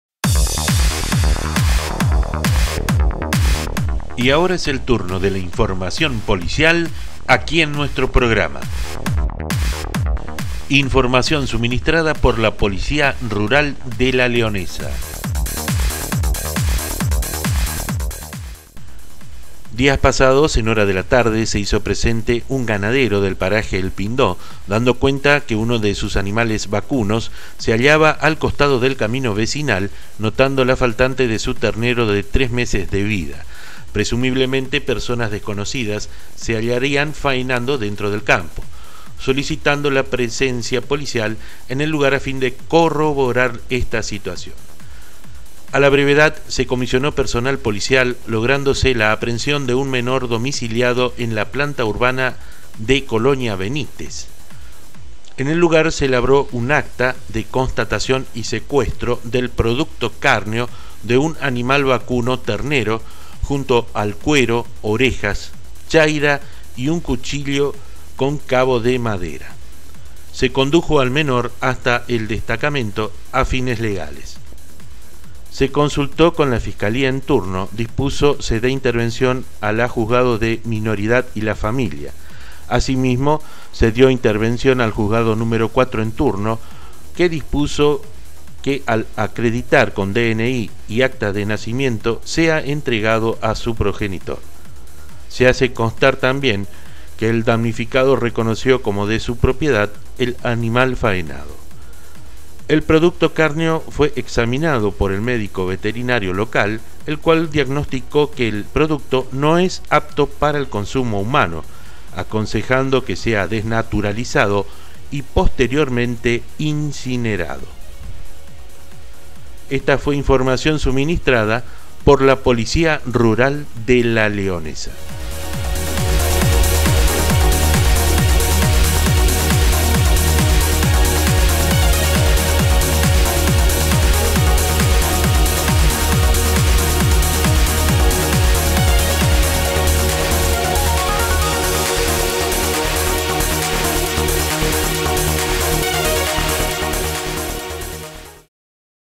OPERATIVO POLICIA RURAL DE LA LEONESA. LUIS BATALLA TV
EXITOSO OPERATIVO DE LA POLICIA RURAL DE LA LEONESA. INFORME POLICIAL SOBRE ABIGEATO. PROGRAMA EL ESPECIAL. LUIS ALBERTO BATALLA TELEVISION.